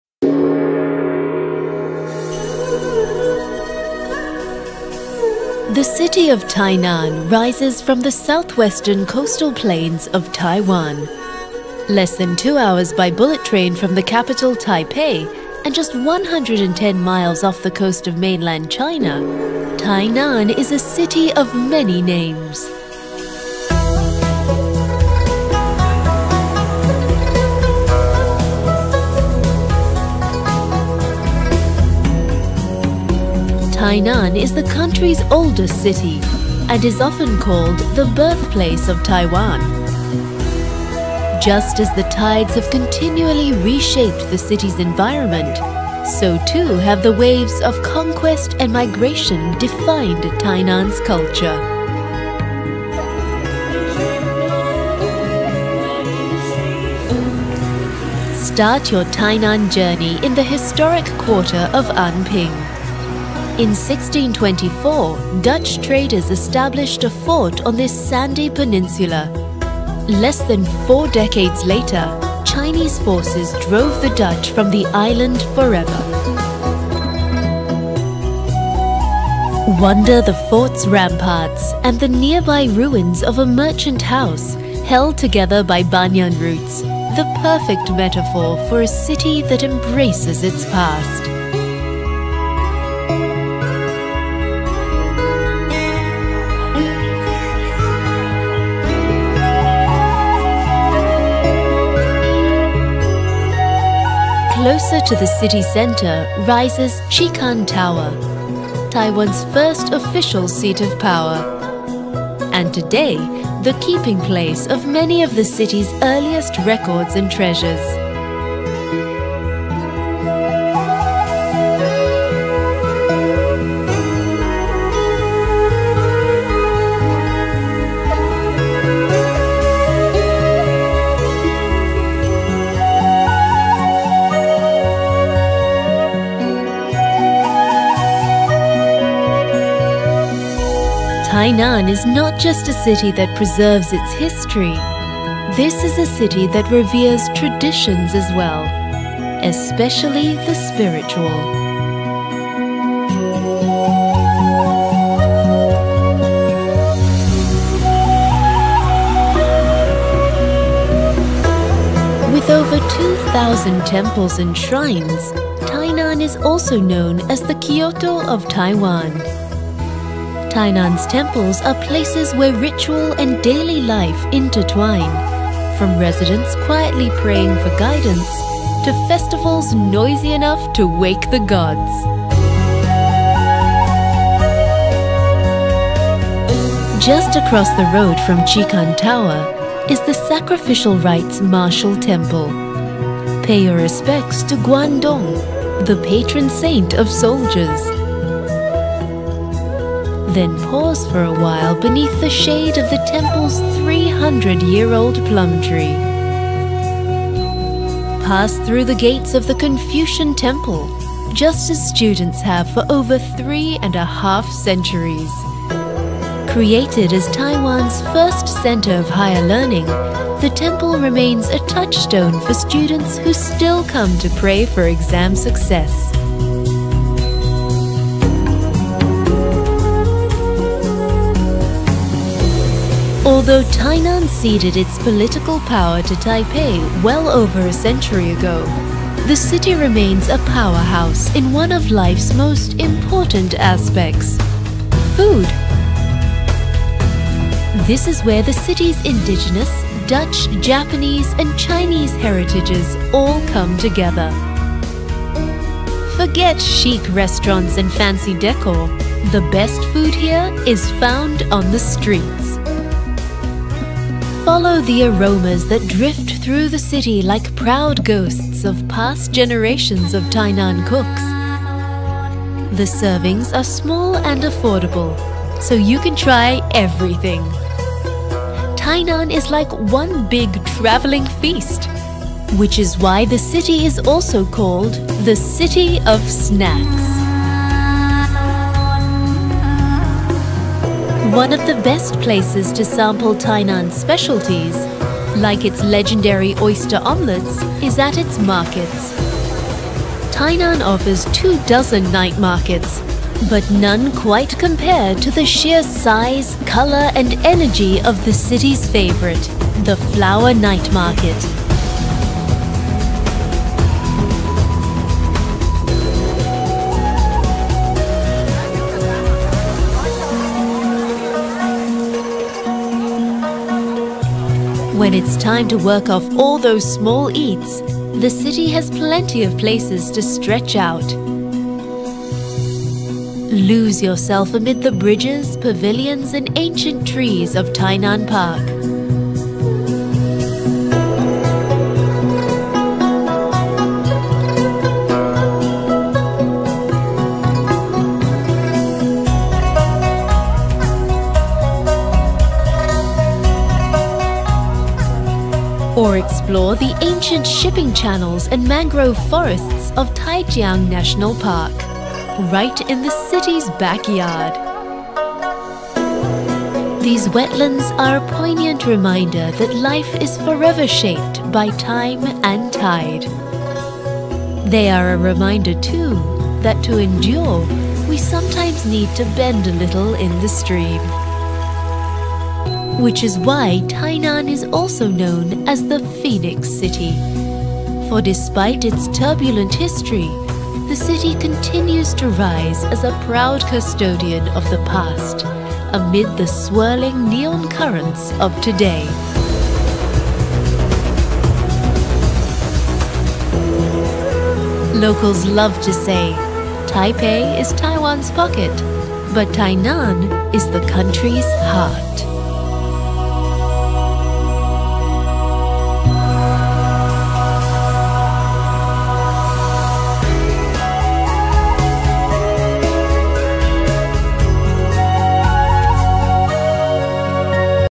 Taiwan - Tainan
Tainan – Embark on a journey to Taiwan's oldest city and immerse yourself in its rich history. Join us as we uncover the most captivating spots in Tainan, inspiring you to craft your own unforgettable itinerary!  When you're ready, explore vacation packages to Tainan

Recognized as "The Birthplace of Taiwan," Tainan beckons you to delve into its local history. Begin by exploring the historic Anping quarter, then make your way to the impressive Chihkan Tower.

Tainan boasts a wealth of history and tradition, showcased through its 2,000 temples and shrines. Discover the Sacrificial Rites Martial Temple, situated across from Chihkan Tower. Pay homage at the altar of Guan Gong, the patron saint of soldiers, and marvel at the 300-year-old plum tree in the courtyard. After a day of cultural immersion, take a leisurely stroll through Tainan Natural Park to glimpse its enchanting mangroves and wetlands.

Feeling peckish? Delight in the culinary offerings of "The City of Snacks," a haven influenced by Chinese, Dutch, and Japanese cuisine. Sample street food around every corner; vendors offer affordable portions, allowing you to savor a diverse range of flavors during your vacation.

For now, we hope you relish watching this travel guide as much as we relished creating it.